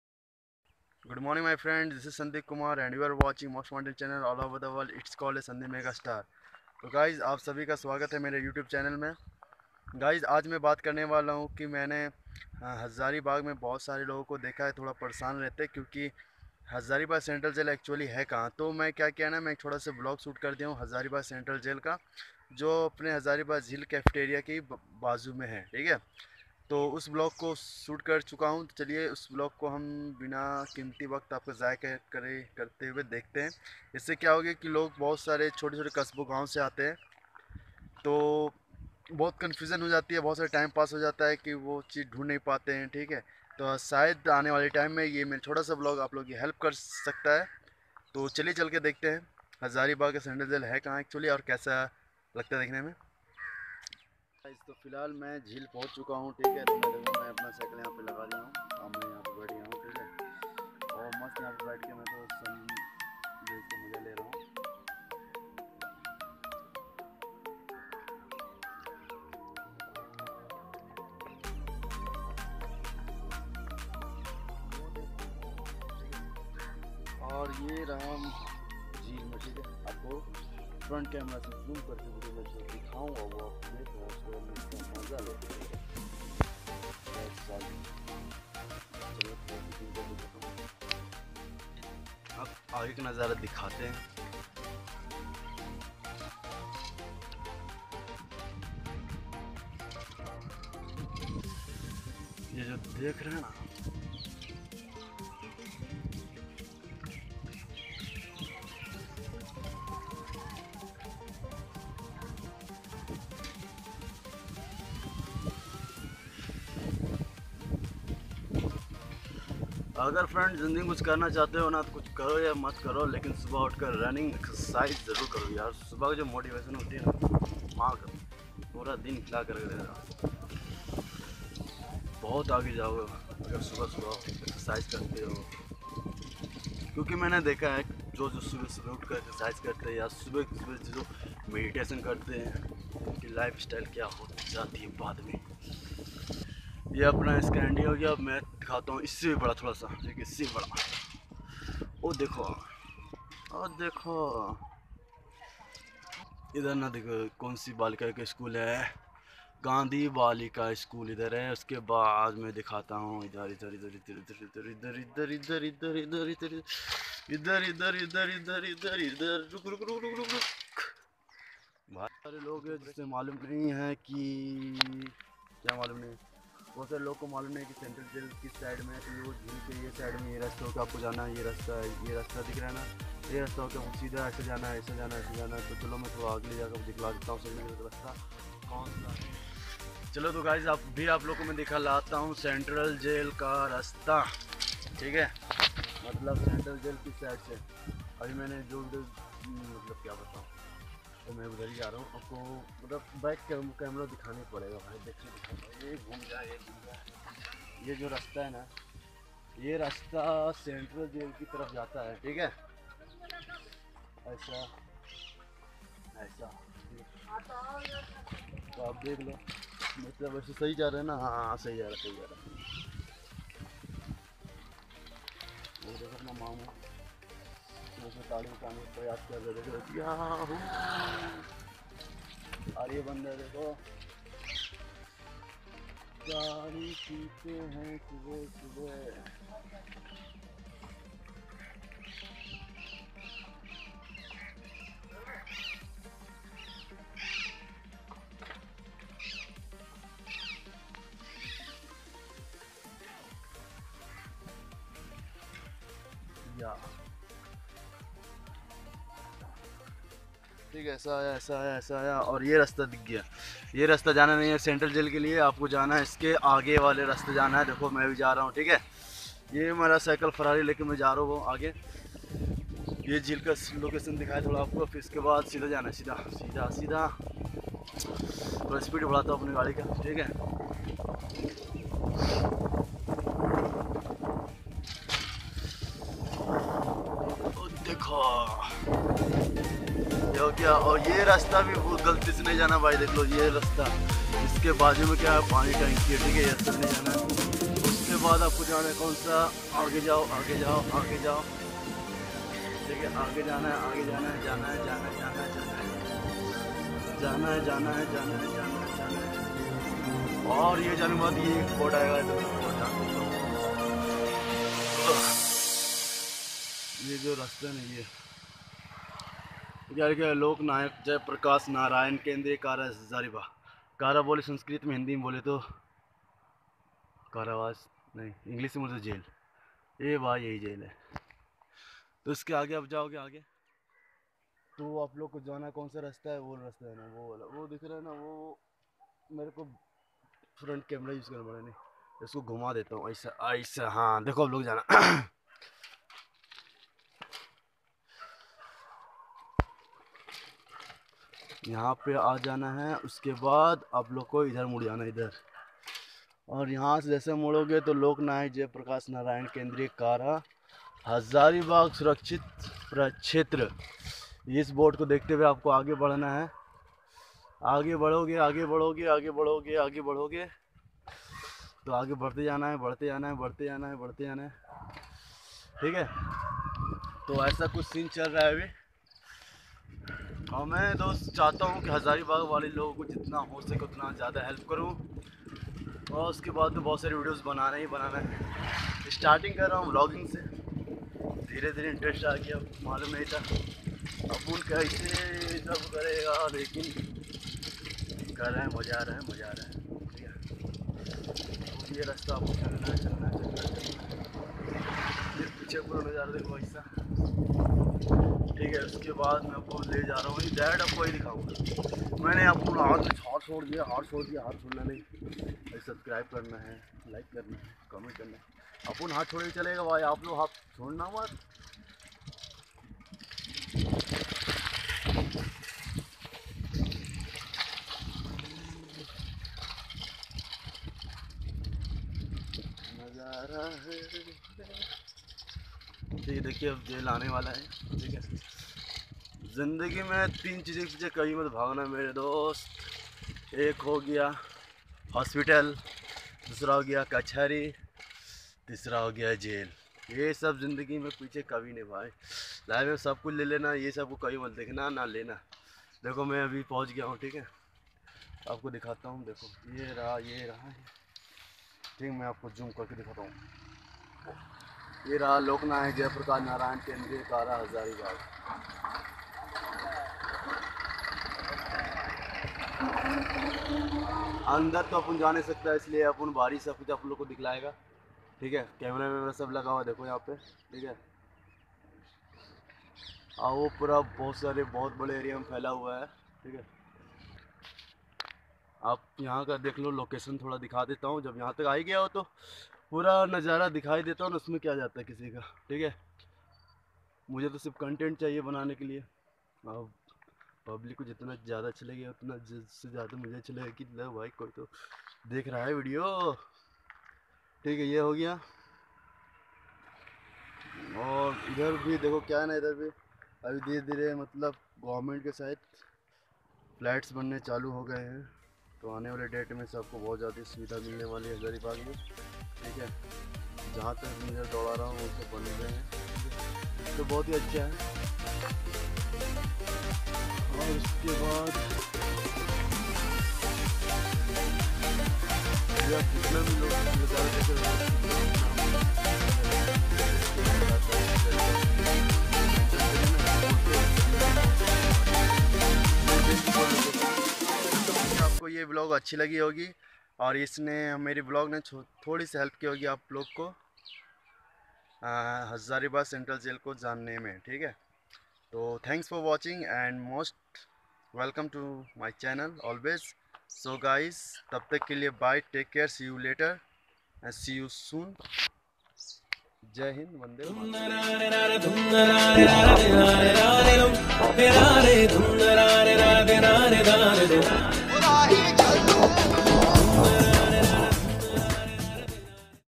WHERE IS THE CENTRAL JAIL OF HAZARIBAG 2018 |SANDIP_MEGASTAR|VLOG=07
Hi,
दोस्तों इस VIDEO में, मैं आपको हज़ारीबाग़ में सेंट्रल जेल कहाँ है ,उसकी पूरी जानकारी दूंगा.
इसलिए आपको ये VIDEO  पूरा अंत तक देखना होगा.
धन्यवाद|
PRESENTED BY-
THIS IS
 "आपका अपना भाई संदीप कुमार"|